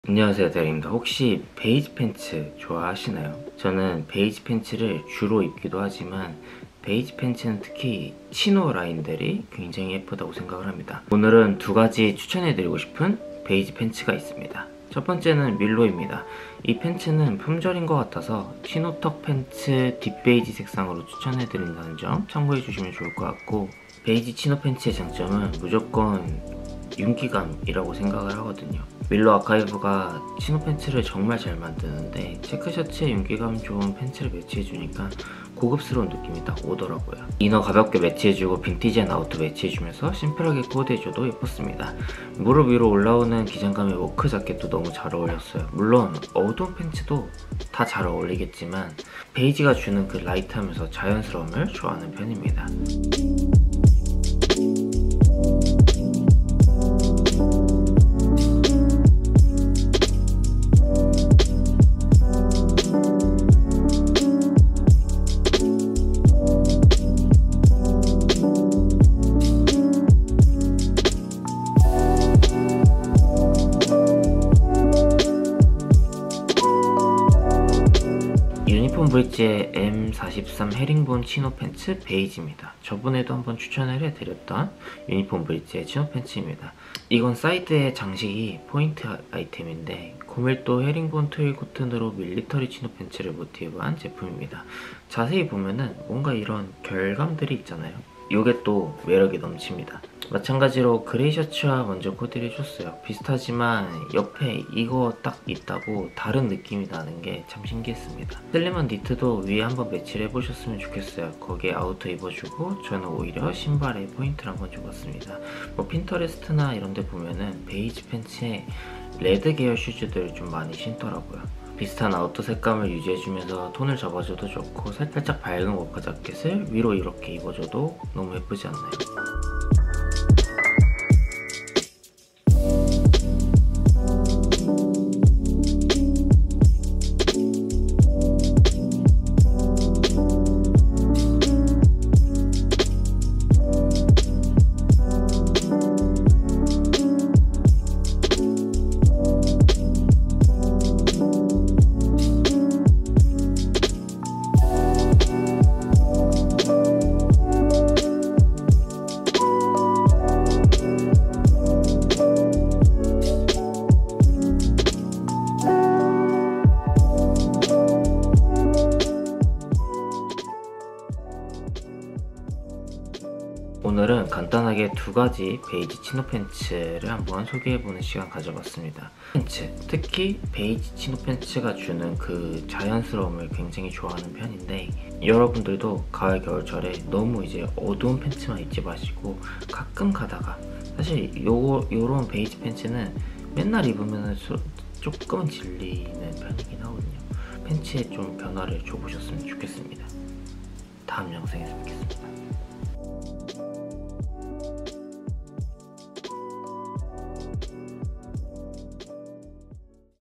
베이지 치노팬츠가 그렇게 예쁘더라구요??
안녕하세요 데릭입니다:)
오늘은 베이지 치노팬츠입니다!!

1.밀로 치노 턱 팬츠 딥베이지
2.유니폼브릿지 헤링본 치노팬츠 베이지

그외 코디
스트라이프 셔츠 : 노클
빈티지 아우터 : 르마드
세미롱기장 자켓 : WGB